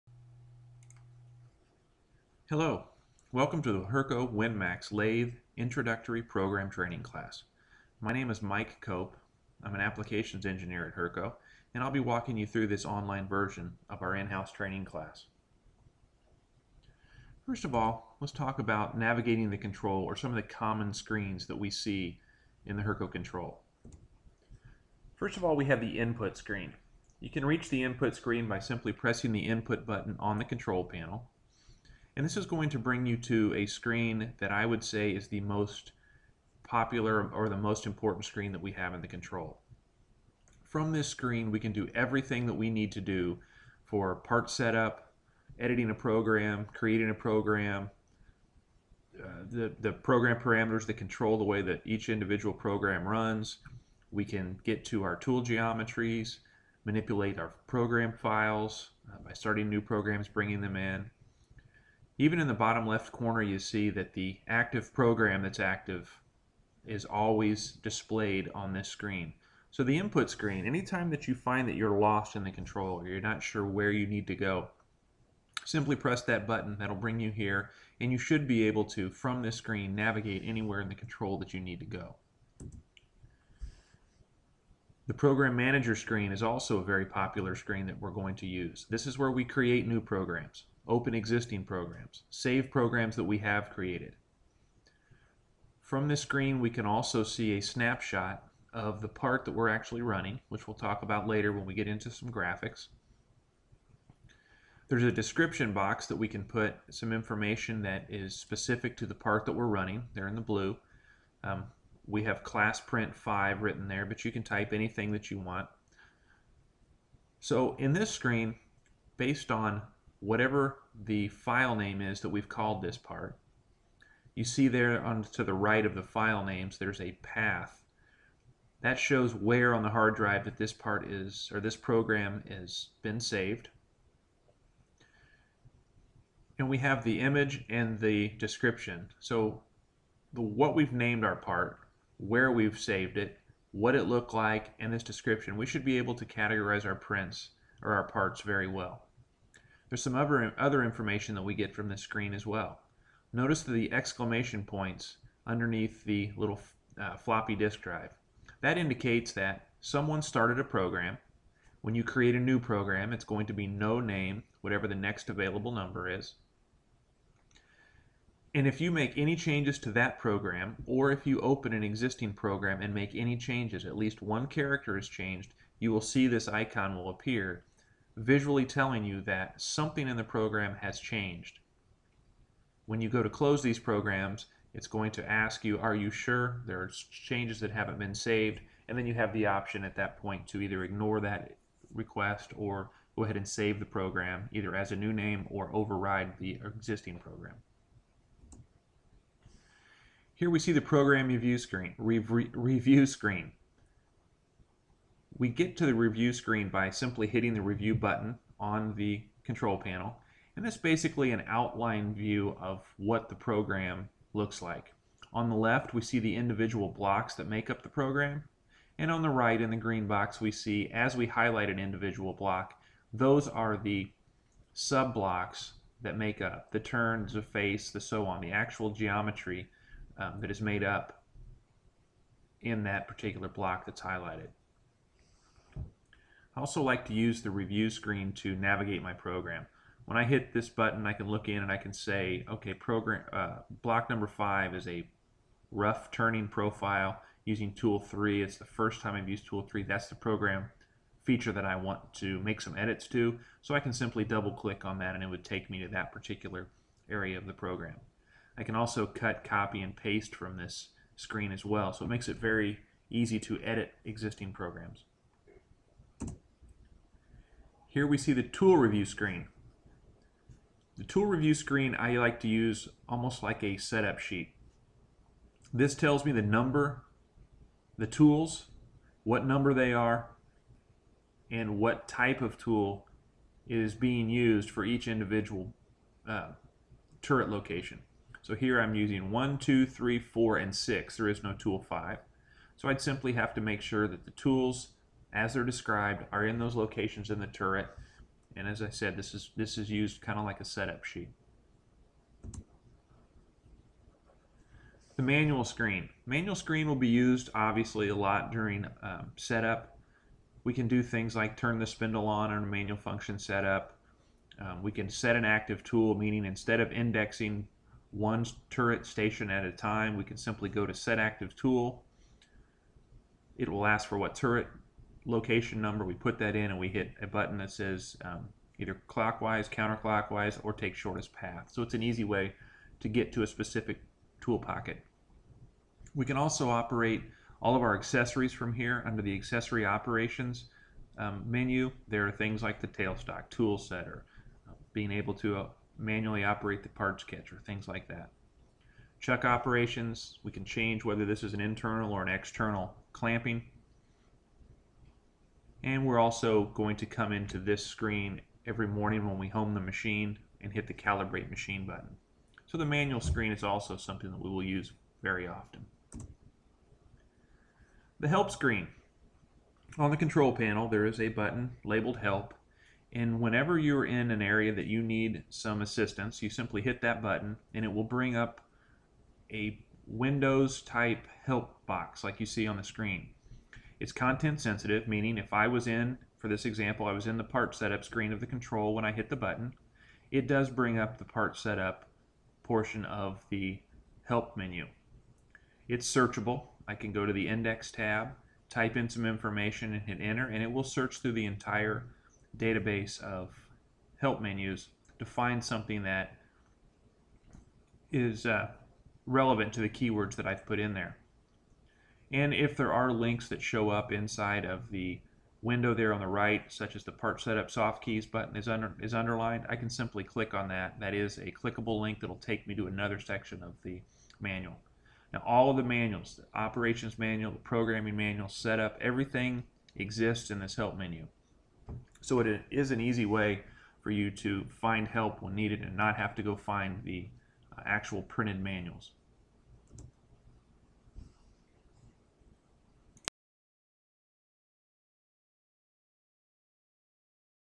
Navigating the Control - Hurco Lathe Training - Max4 Version 9
Some helpful information for navigating the Hurco WinMax lathe control.